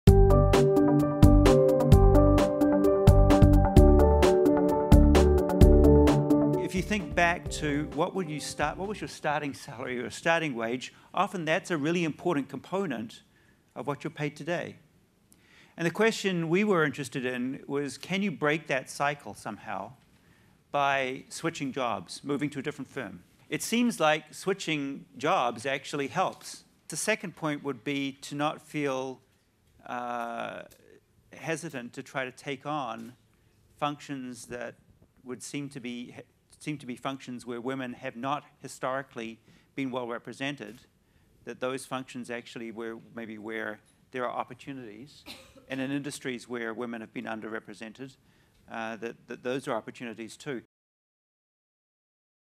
Get Paid What You’re Worth: The Benefit of Switching Jobs
Paul M. Healy, James R. Williston Professor of Business Administration and Senior Associate Dean for Faculty Development at Harvard Business School discusses the benefits of switching jobs during the 2015 Get Paid What You’re Worth panel.